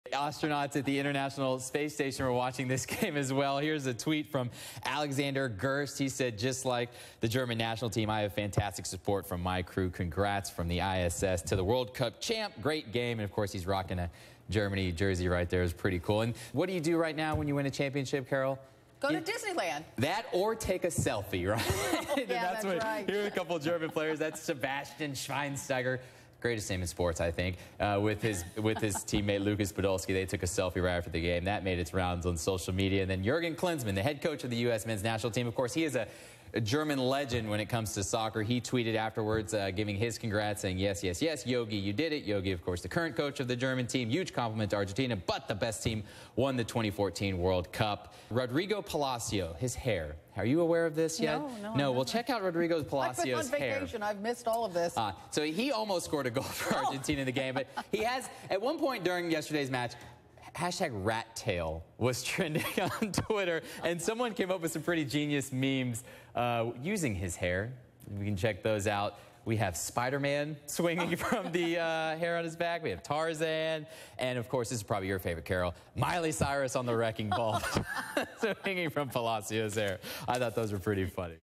Must see World Cup trending photos
Argentinian player Rodrigo Pacio's hairstyle created a social media buzz during the final World Cup game against Germany.